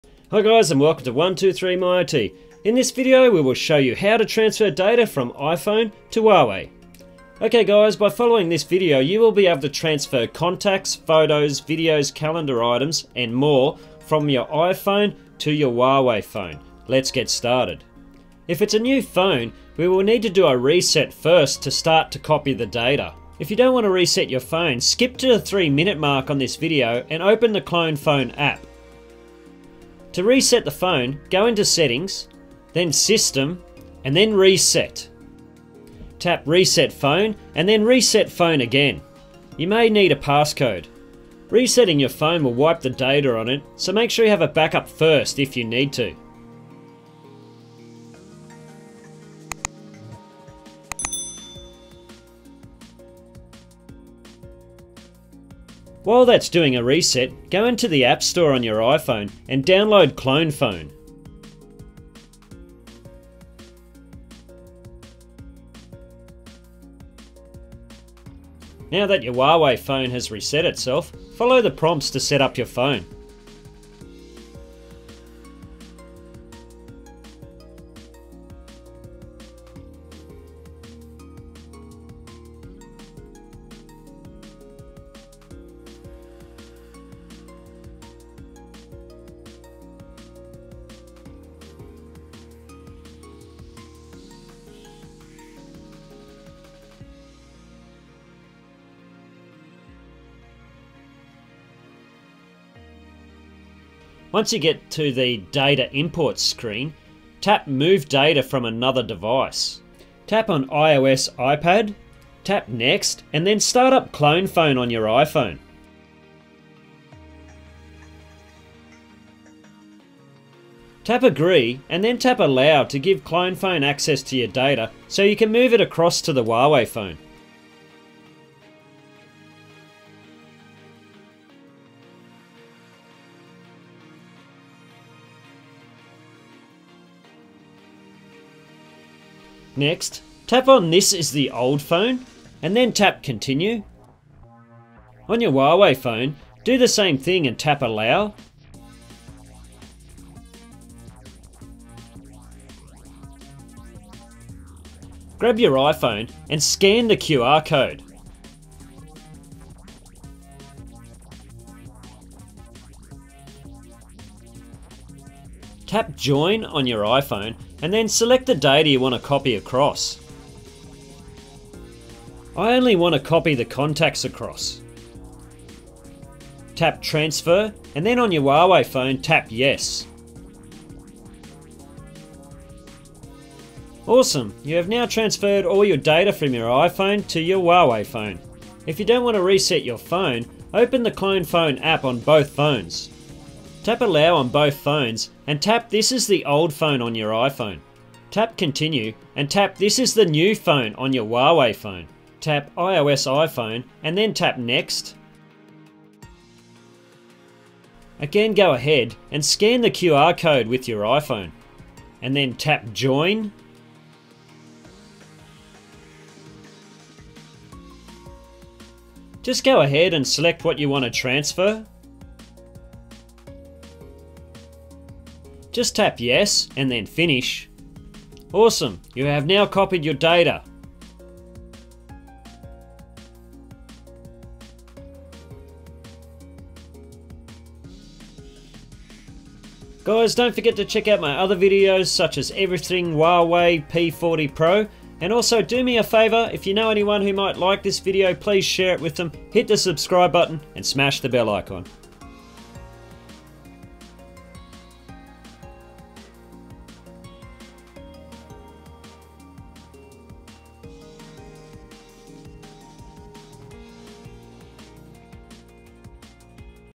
Transfer Data iPhone to Huawei
In this video we show you how to Transfer Data iPhone to Huawei. This is helpful if you want to transfer contacts, Photos, Videos, Calander items and more from your iPhone to your Huawei.